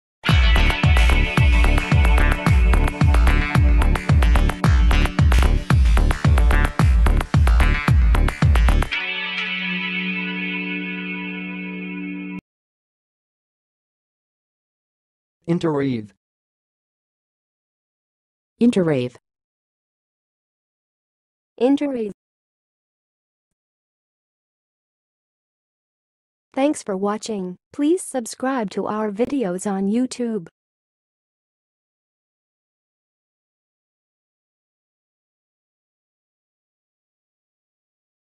How to say "interwreathe"! (High Quality Voices)
Watch in this video how to say and pronounce "interwreathe"!